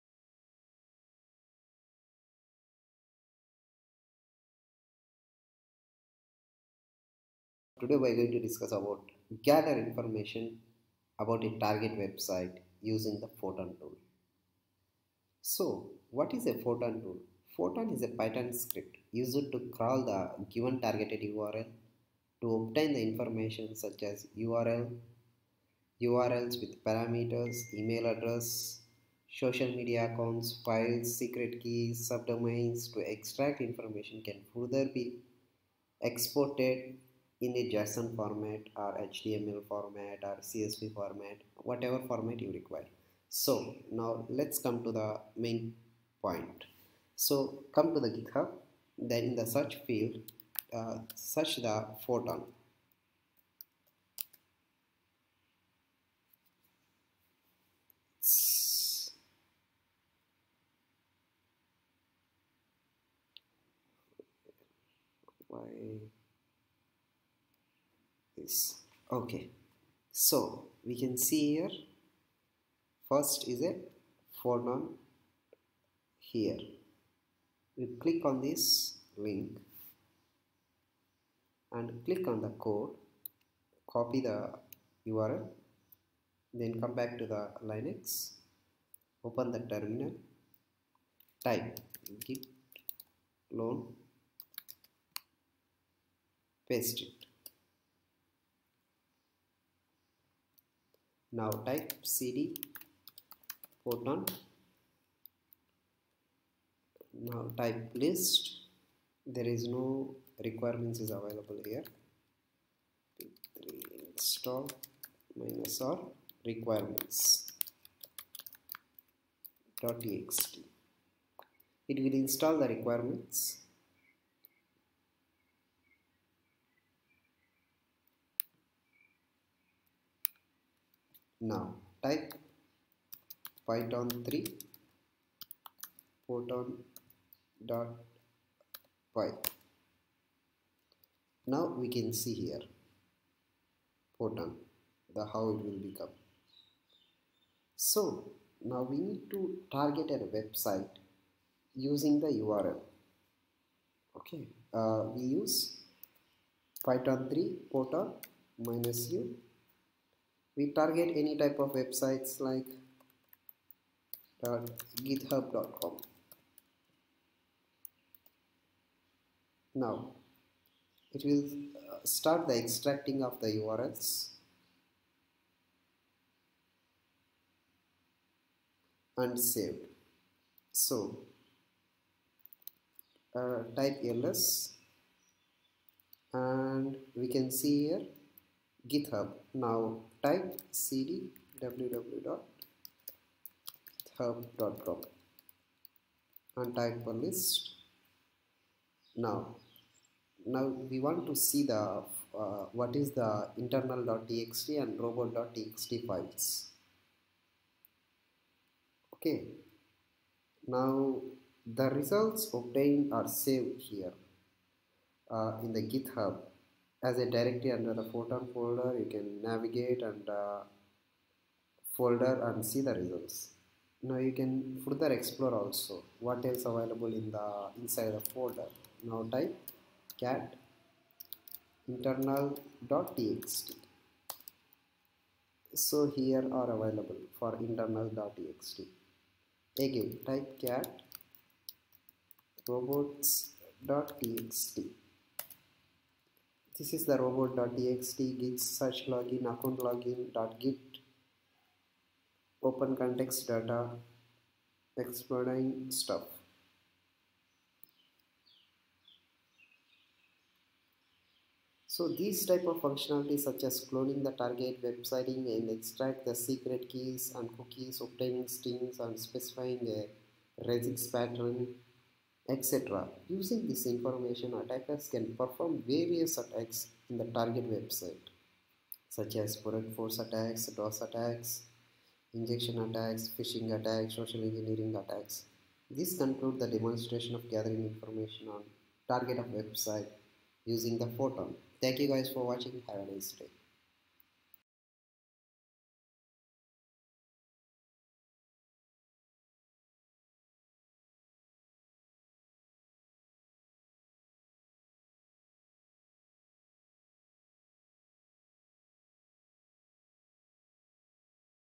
Gather Information about a Target Website using Photon
Key Features
Data Extraction

Photon can extract the following data while crawling:

    Intel (emails, social media accounts, amazon buckets etc.)
    Files (pdf, png, xml etc.)
    Secret keys (auth/API keys & hashes)
    JavaScript files & Endpoints present in them
    Strings matching custom regex pattern
    Subdomains & DNS related data

The extracted information is saved in an organized manner or can be exported as json.

Flexible

Control timeout, delay, add seeds, exclude URLs matching a regex pattern and other cool stuff. The extensive range of options provided by Photon lets you crawl the web exactly the way you want.
Genius

Photon's smart thread management & refined logic gives you top notch performance.

Still, crawling can be resource intensive but Photon has some tricks up it's sleeves. You can fetch URLs archived by archive.org to be used as seeds by using --wayback option.
Plugins

    wayback
    dnsdumpster
    Exporter